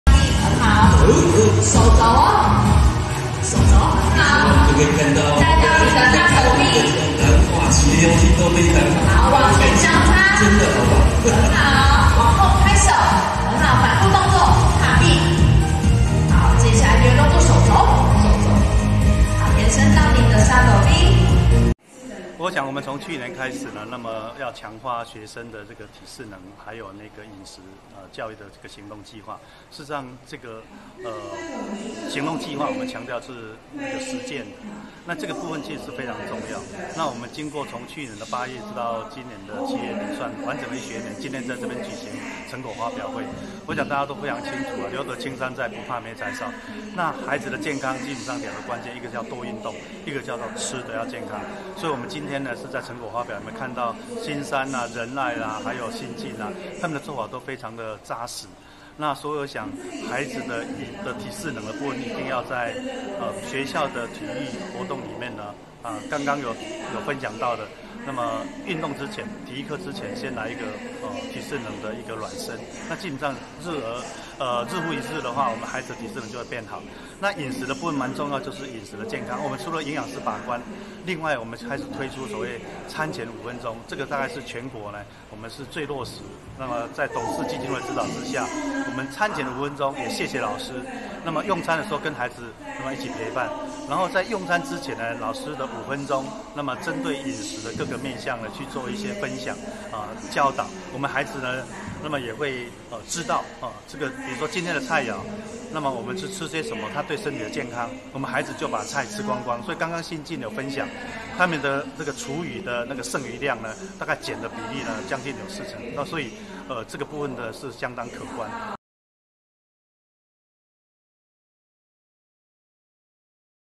台南中小學將推創意健身操 3分18秒相當跑步5-10分鐘
幫助台南學子養成規律運動與正確飲食習慣，台南市開發一套創意健身操，下學期起將在全市中小學推動，全長3分18秒的健身操，約可消耗200-300卡路里，對心肺功能提升相當於跑步5-10分鐘，一般民眾也可跟著做。